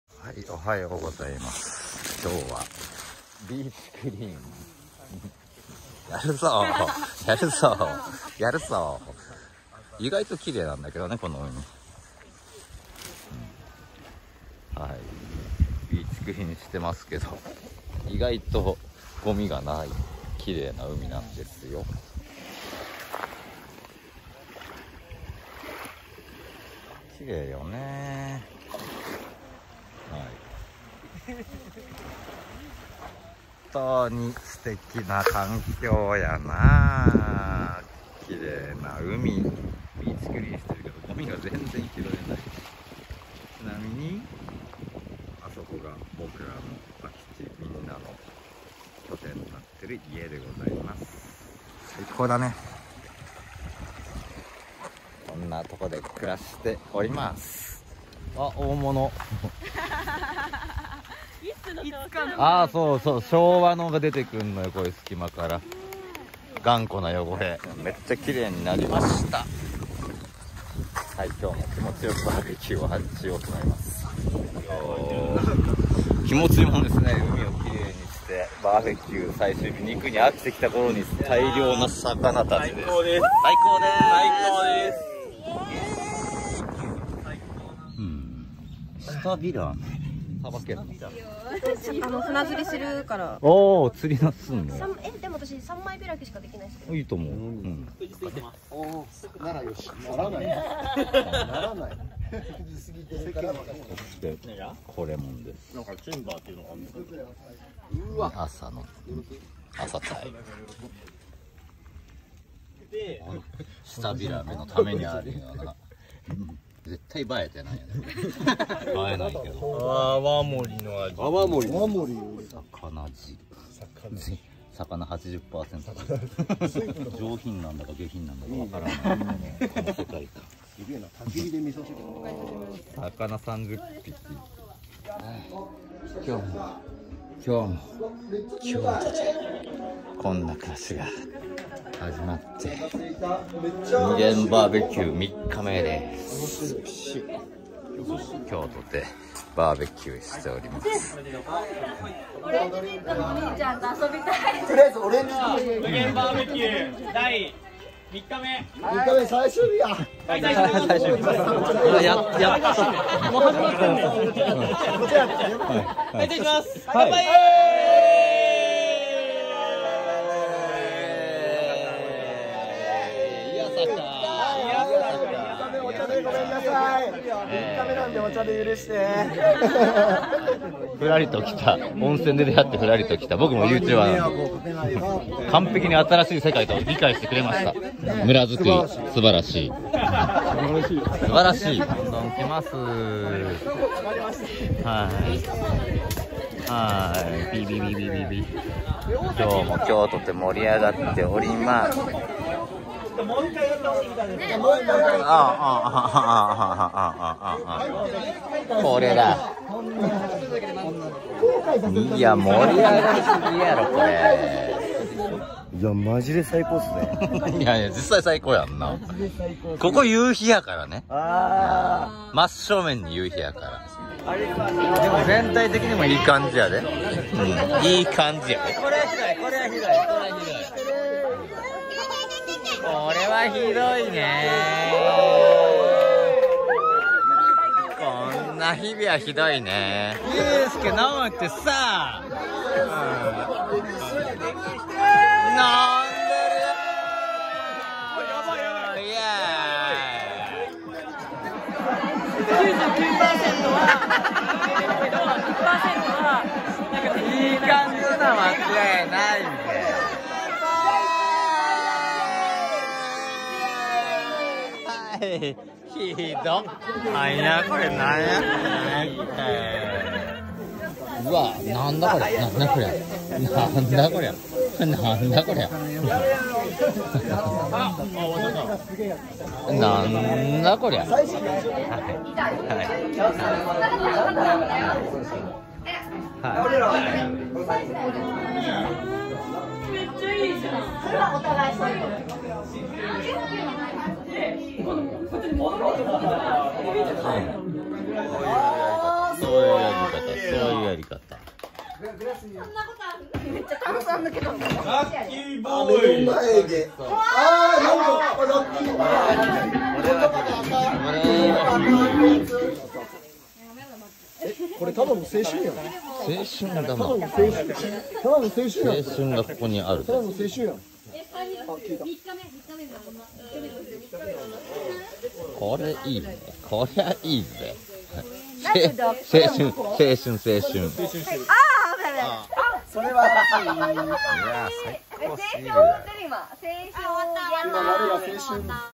【愛知の村づくり】〈DAY53〉『無限BBQ - DAY 3』
村づくり の拠点となる家と土地(空き地)にて、みなさんとの現場感と懇親を深めるための3日間のBBQ！！

　最終日も大盛り上がり❗️❗️❗️

⭕️ 愛知の村づくり な日々、配信中！